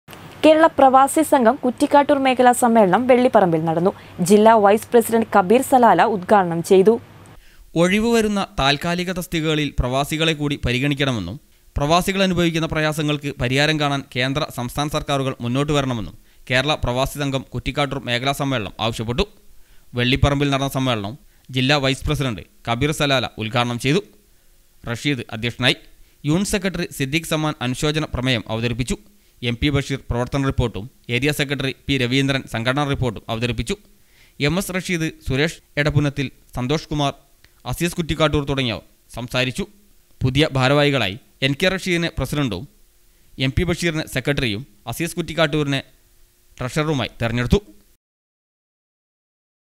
കേരള പ്രവാസി സംഘം കുറ്റിക്കാട്ടൂർ മേഖല സമ്മേളനം വെള്ളിപറമ്പിൽ നടന്നു
കേരള പ്രവാസി സംഘം കുറ്റിക്കാട്ടൂർ മേഖല സമ്മേളനം വെള്ളിപറമ്പിൽ നടന്നു. ജില്ല വൈസ് പ്രസി: കബീർ സലാല ഉദ്ഘാടനം ചെയ്തു